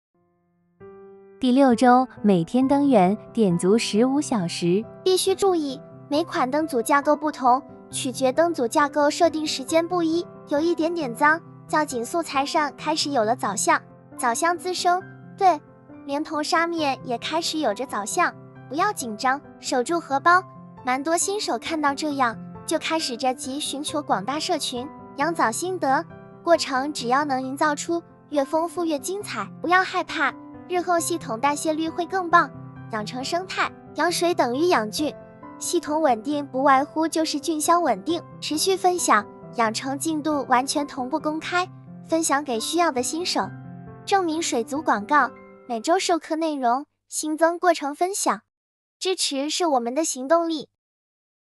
養成計劃海水系統開缸過程
蠻多新手面臨開缸過程，總會遭受很多打擊，除了面對滋生整個水族箱有著藻相慘不忍睹，開始懷疑到底是否還要做什麼

這部短片可以帶來給你一點強心丸，只要跟著步驟我們一起陪你撐過去，與藻共生，讓生態豐富起來